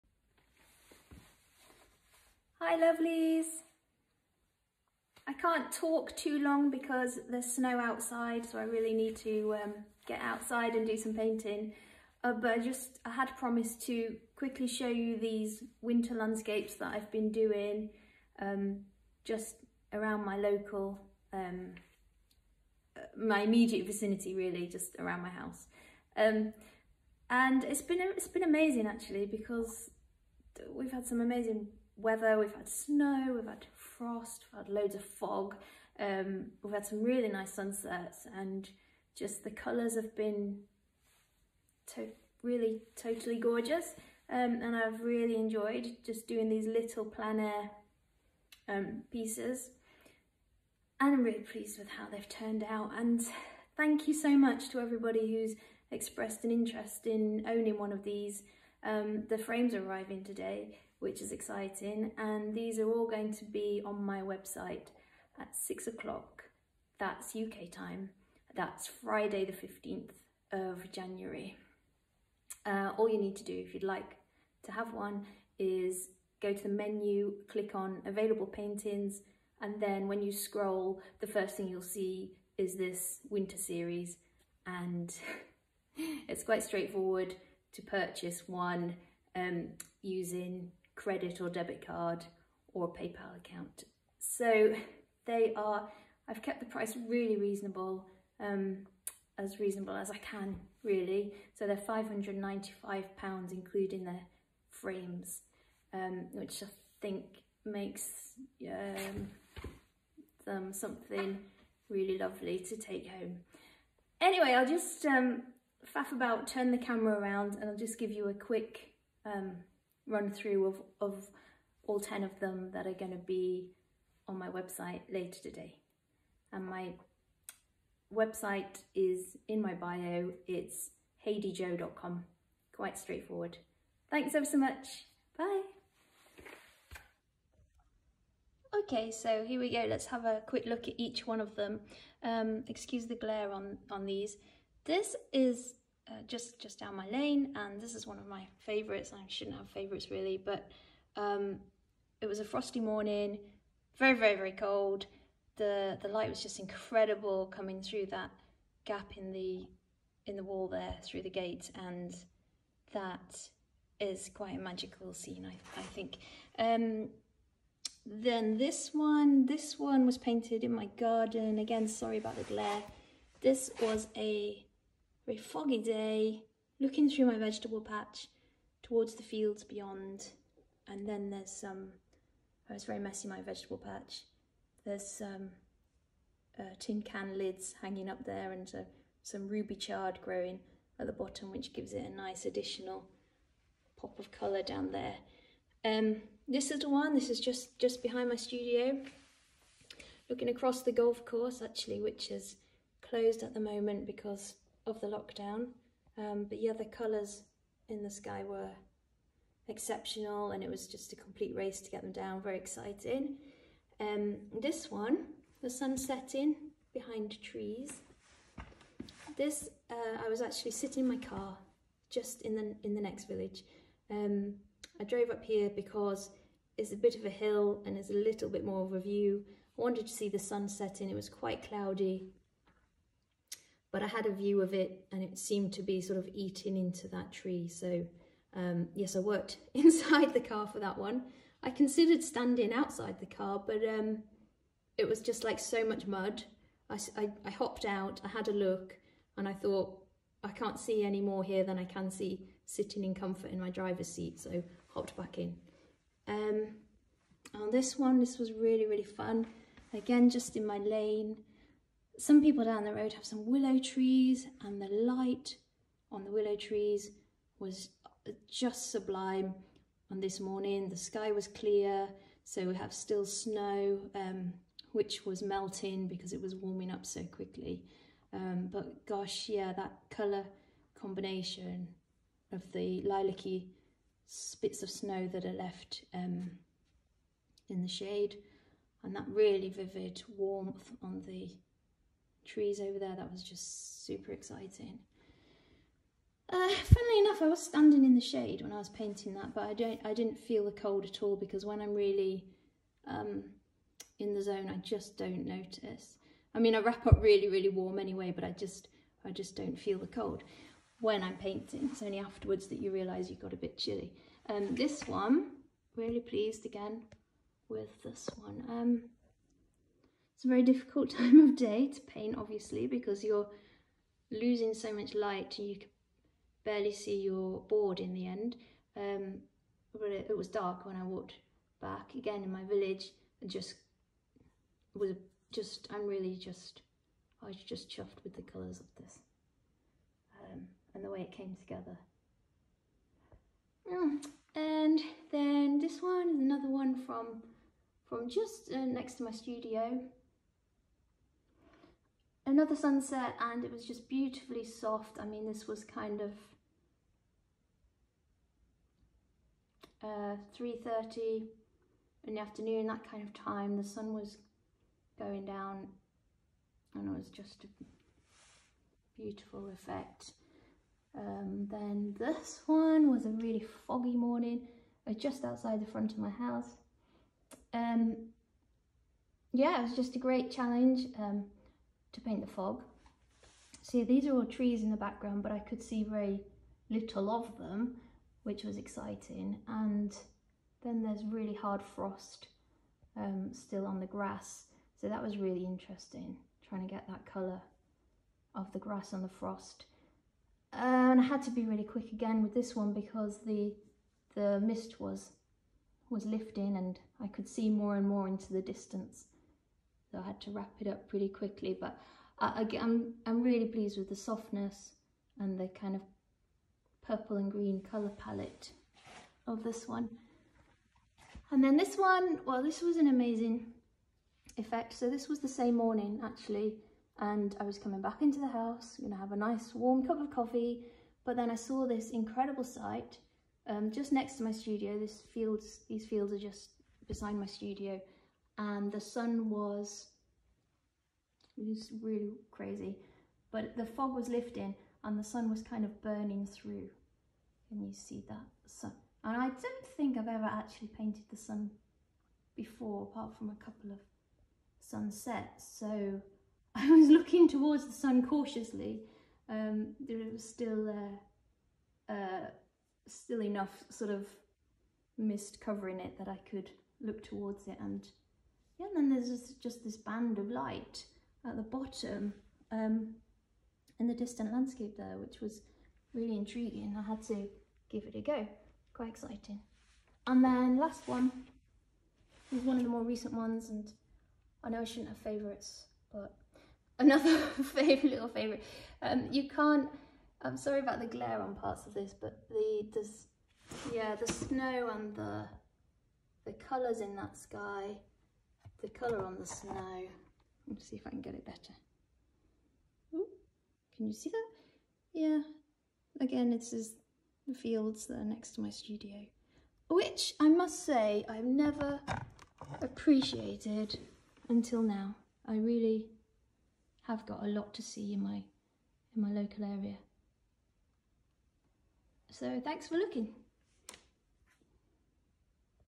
Winter landscape collection
Thought you’d like to see the collection of little plein air winter landscapes that I’ve been working on. Take care x